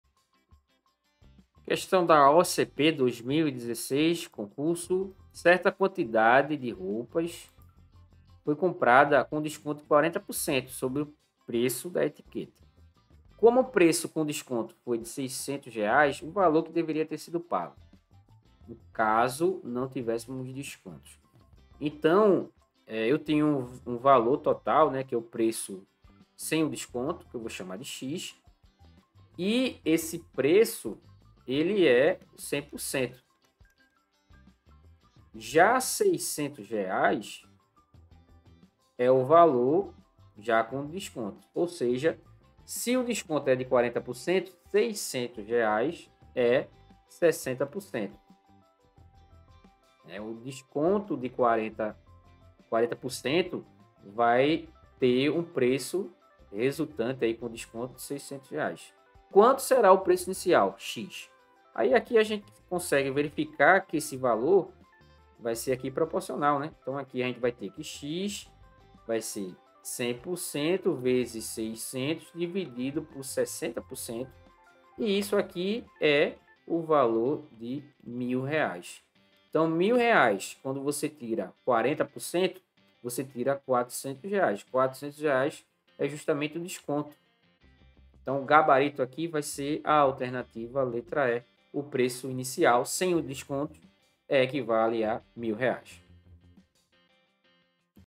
(AOCP-2016) Certa quantidade de roupas foi comprada com um desconto de 40% sobre o preço da etiqueta
(AOCP-2016) Certa quantidade de roupas foi comprada com um desconto de 40% sobre o preço da etiqueta. Como o preço com desconto foi R$ 600,00, o valor que deveria ter sido pago caso não tivéssemos os descontos é:
A) R$240.
B) R$360.
C) R$800.
D) R$980.
E) R$1000.